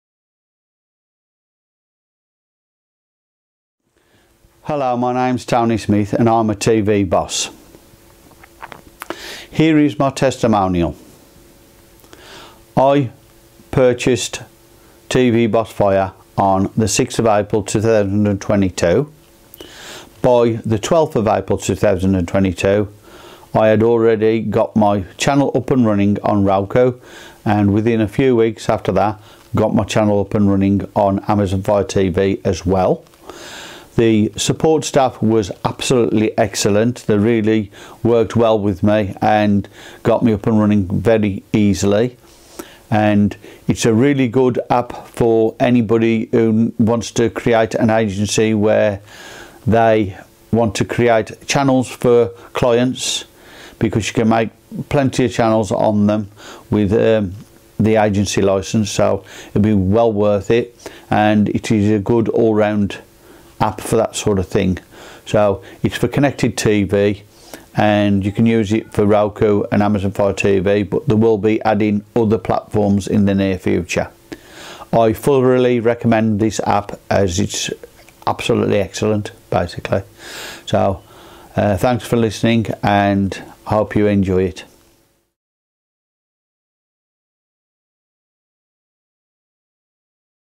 TVBoss Fire Testimonial
This is a testimonial of TVBoss Fire as I was able to use it really easily and found it a fantastic app!